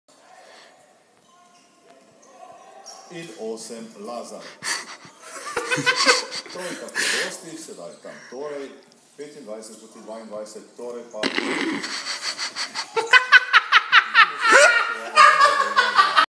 Borči getting OWNED
Bor getting crossed. Noob basketball gets his ankles broken.